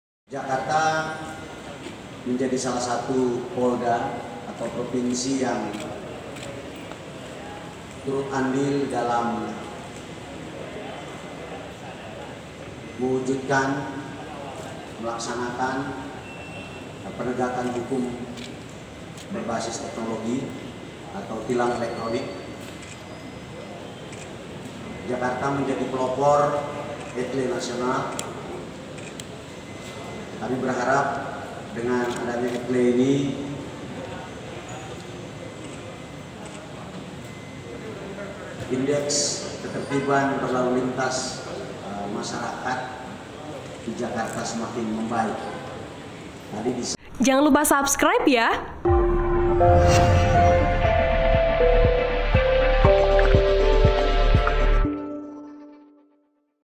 Kapolda Metro: Jakarta Jadi Pelopor ETLE Nasional
WARTA KOTA, SEMANGGI
Kapolda Metro Jaya Irjen Fadil Imran mengatakan Jakarta menjadi salah satu Polda atau provinsi yang
turut andil di dalam mewujudkan, melaksanakan dan menegakan hukum berbasis teknologi atau tilang elektronik.

Karenanya kata dia Jakarta adalah kota yang lebih dulu menereapkan ETLE di Indonesia dan merupakan pelopor ETLE Nasional.

"Jakarta menjadi pelopor ETLE nasional. Kami berharap dengan adanya ETLE ini, indeks ketertiban berlalu lintas masyarakat di jakarta semakin membaik," kata Fadil usai menyaksikan peluncuran ETLE Nasional oleh Kapolri secara virtual di Mapolda Metro Jaya, Selasa (23/3/2021).

Data dari Dirlantas Polda Metro Jaya kata Fadil di titik yang ada kamera ETLE terjadi penurunan signifikan pelanggaran lalu lintas.

Video Jurnalis  :  Budi Sam Law Malaw